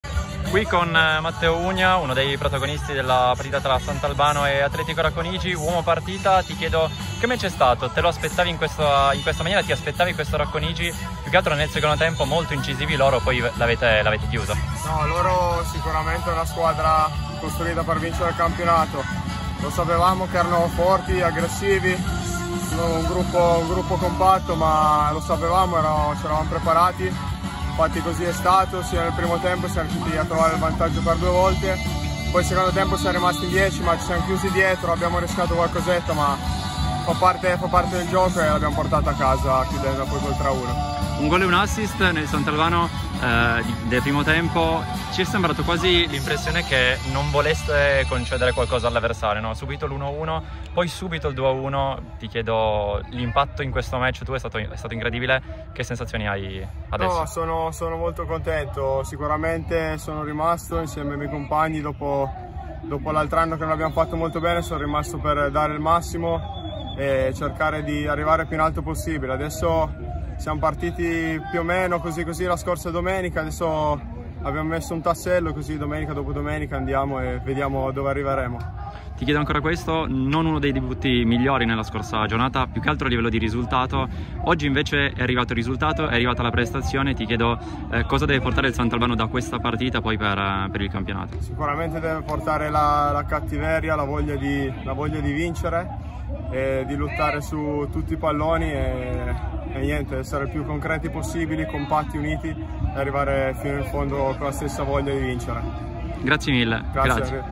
Le parole di Matteo Unia dopo il tris del Sant'Albano
Primo successo stagionale per il Sant'Albano che al "Don Eandi" cala il tris sull'Atletico Racconigi nella seconda giornata di Prima Categoria.
Intervista a cura di Matteo Pipino.
Seguici sui nostri profili per restare aggiornati sugli articoli sportivi e non solo!